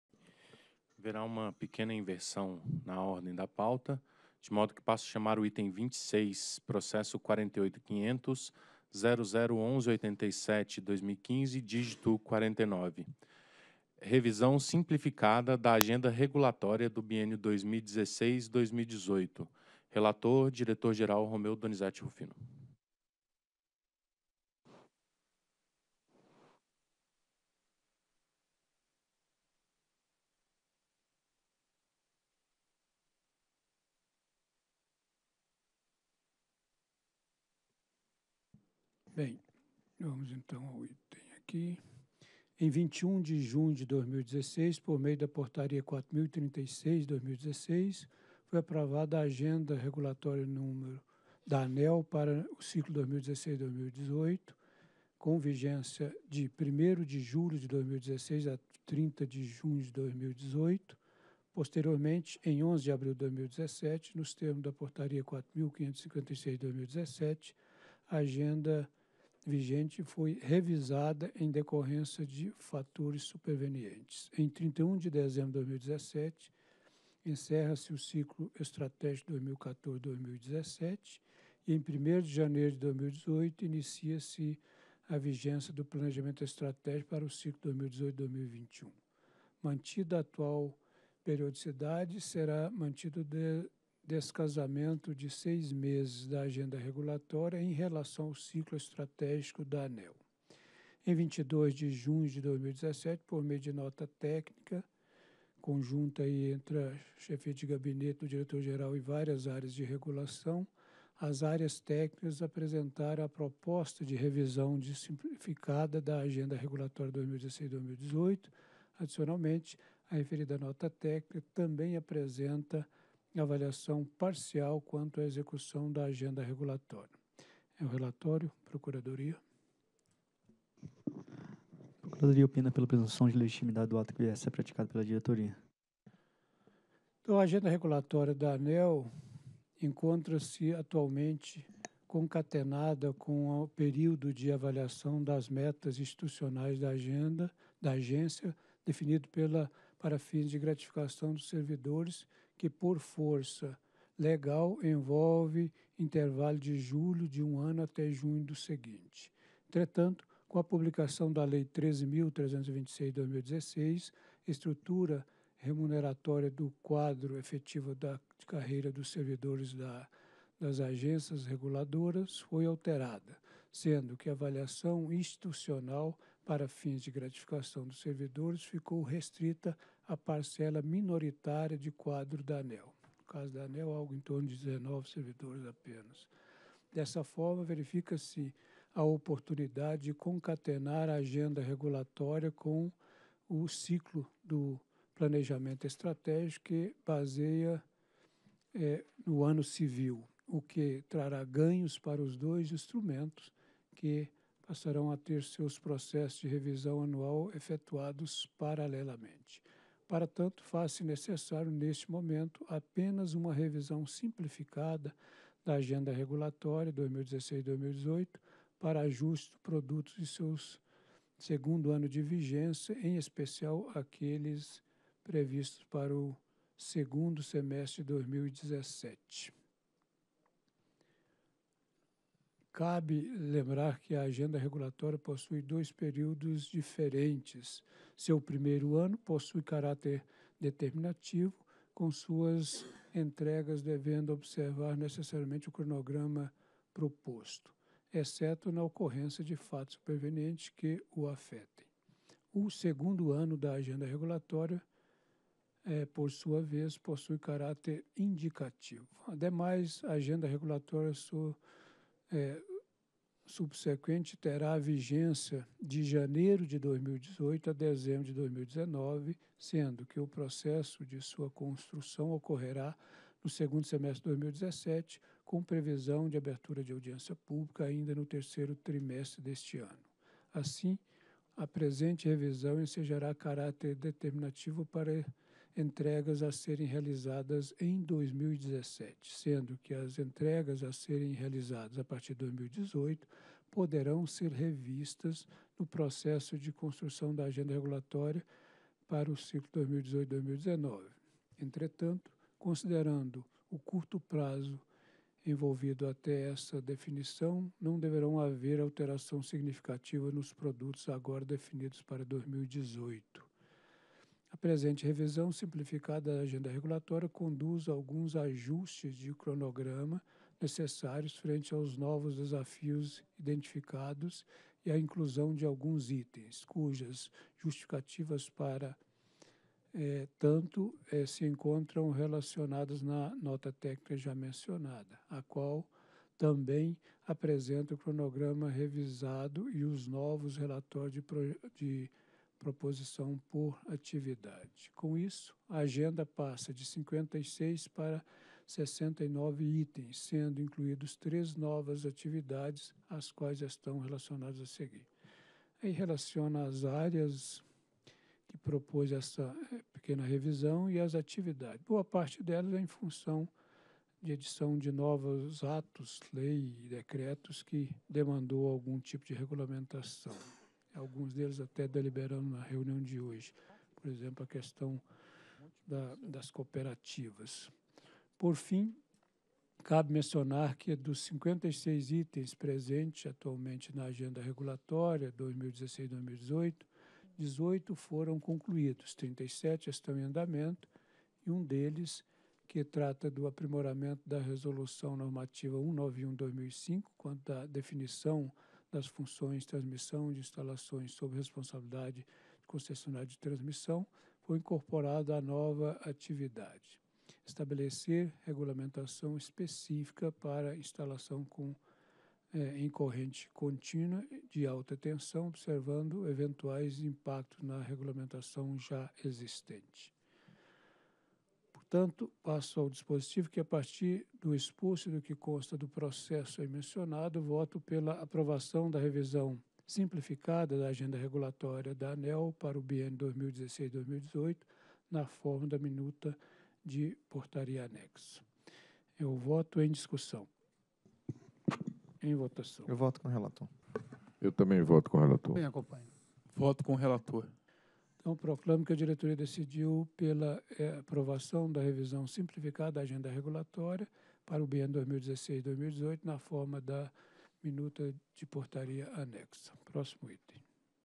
Item 26 - 23ª Reunião Pública Ordinária ANEEL 27/06/2017
Item 26. Processo: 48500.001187/2015-49. Assunto: Revisão Simplificada da Agenda Regulatória do biênio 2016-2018. Relator: Diretor-Geral Romeu Donizete Rufino.